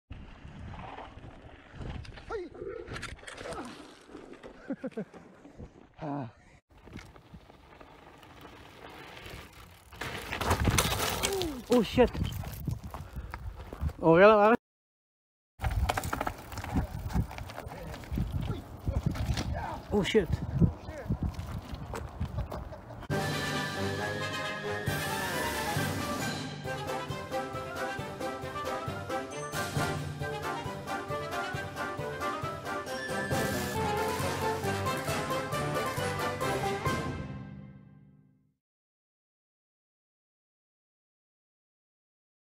KelCrash 2020
Wish I had footage of all my Crashes ... but here are 4 of the first ones ... lol